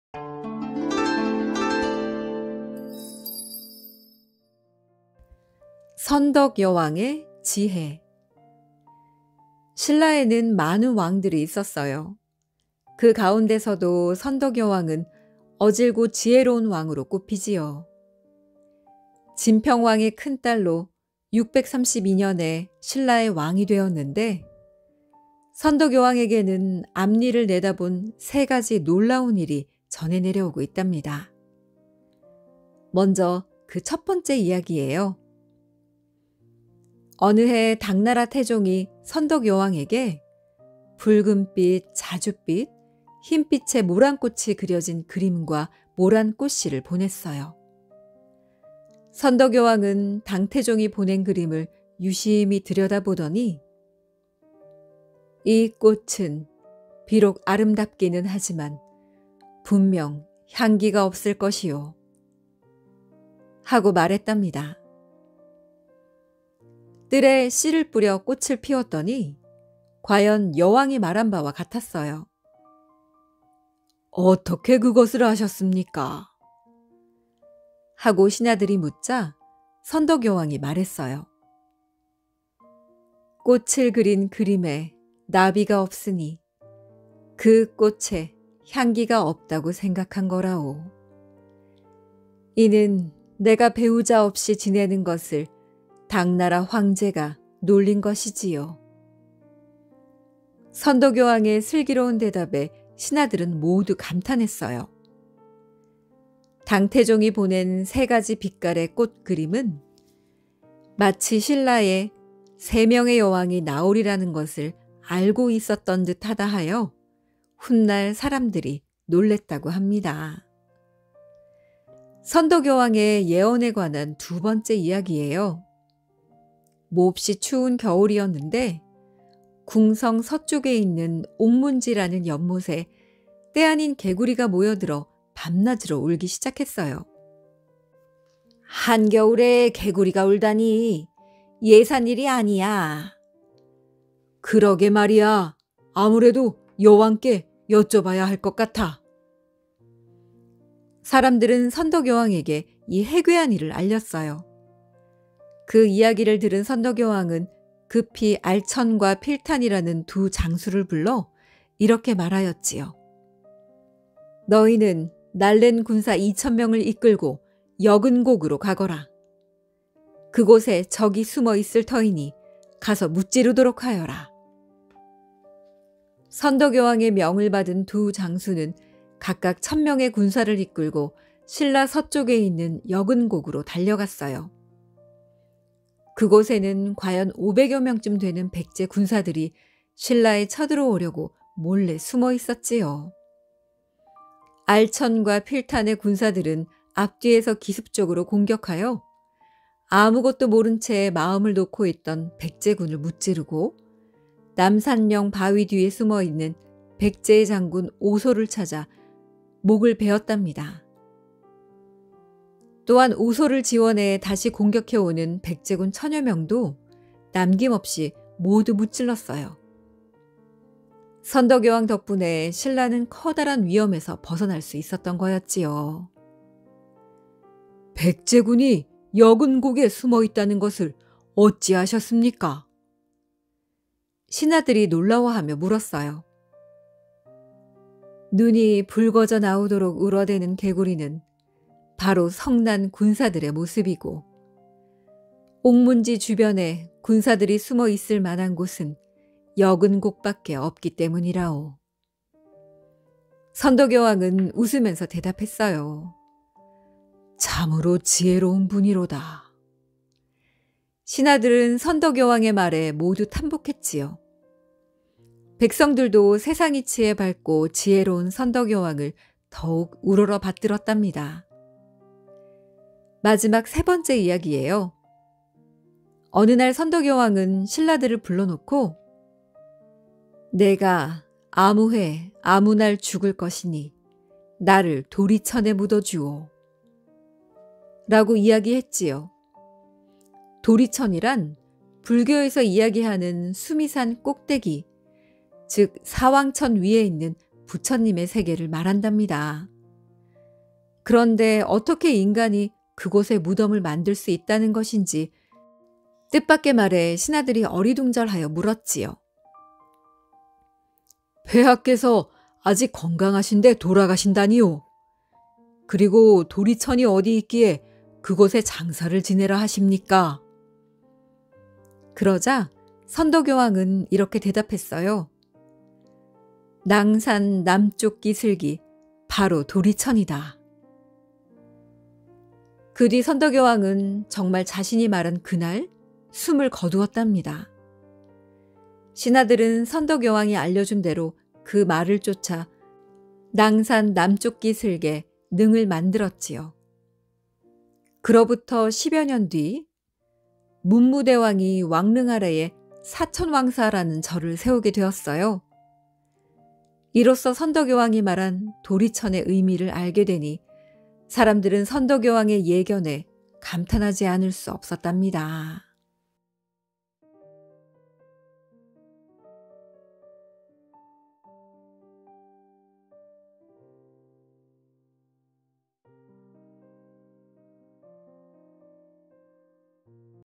[옛이야기]한겨울 개구리 소리 | 선덕여왕의 신통한 예지력 | 인물신화
한겨울에 개구리가 울어대자 신하들은 의아해하며 선덕여왕에게 이야기합니다.
그 이야기를 들은 선덕여왕은 신라 서쪽 여근곡에 군사들을 보내게 되는데요..
과연 어떤 일이 일어났을까요?
▬▬▬▬타이틀음악▬▬▬▬
선덕여왕의 신통한 예지력

(도서정보)
미리 읽어보는 우리 인물 일화 / 글 김경화 그림 이진경